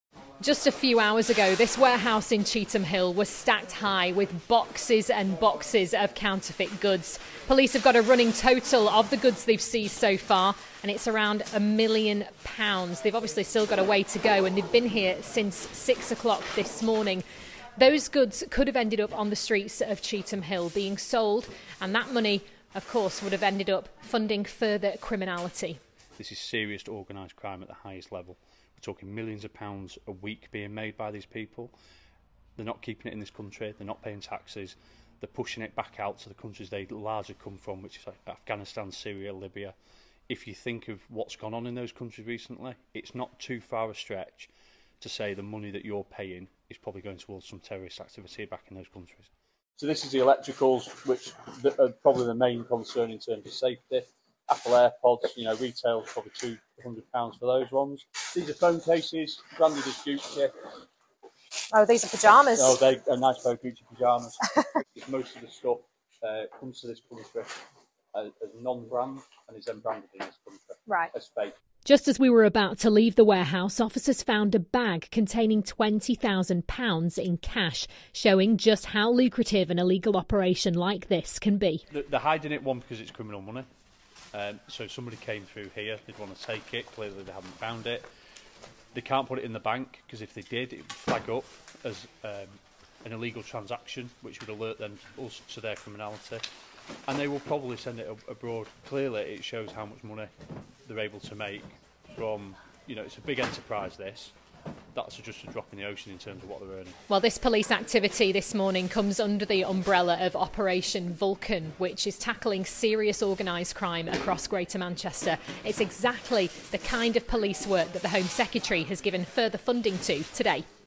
Police raid finds a MILLION POUNDS of counterfeit goods
Cheetham Hill was once branded the counterfeit capital of the U.K. It’s now a ghost town as police continue their crackdown on organised crime.

Our regional correspondent Victoria Glover was exclusively invited to see officers raid a warehouse housing more than a million pounds worth of goods